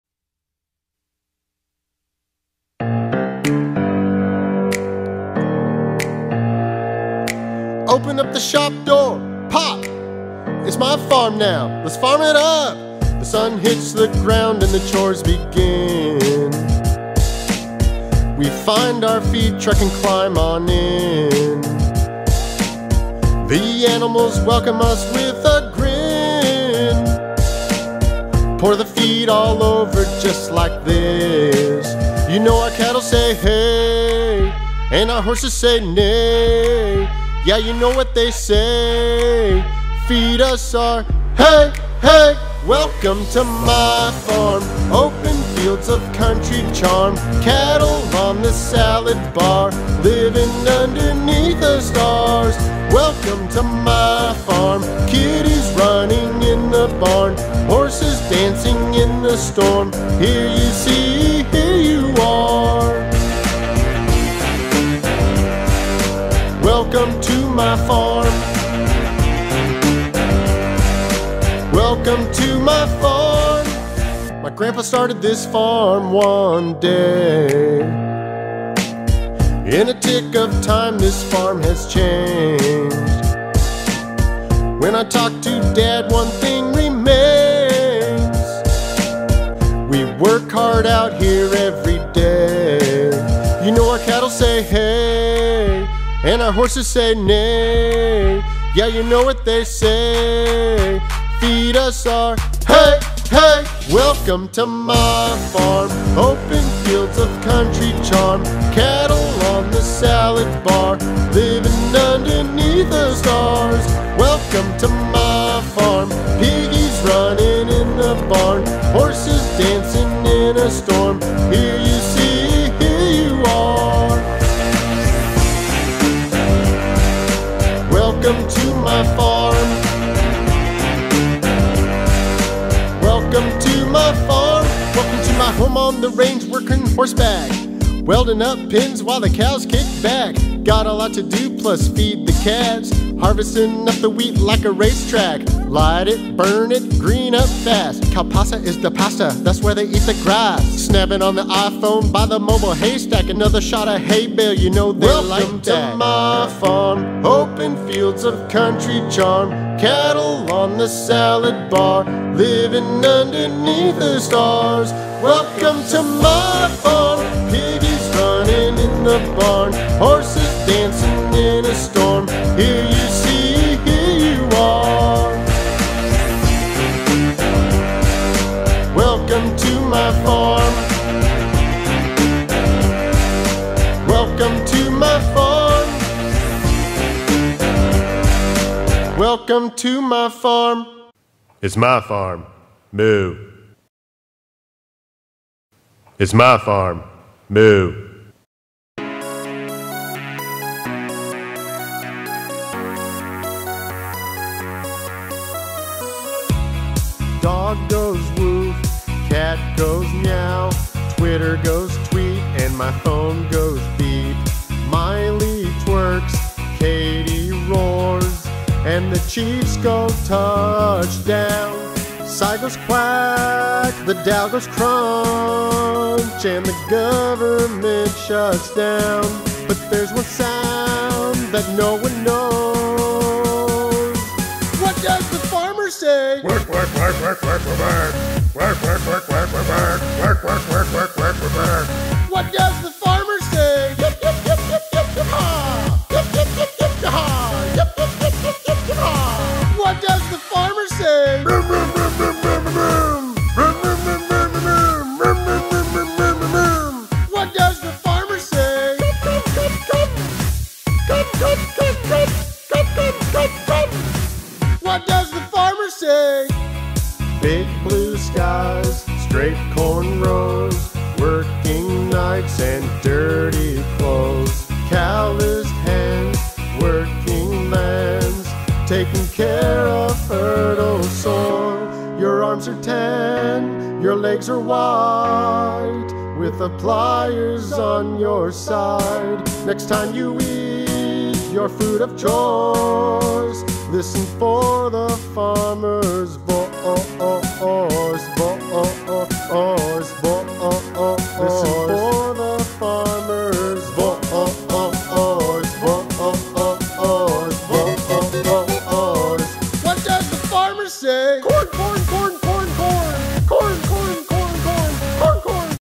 A Day in the Life of My Dad - Moviemaking Merit Badge
Ground Crew: Hoss Johnson
Actor: My dad, Brandon Johnson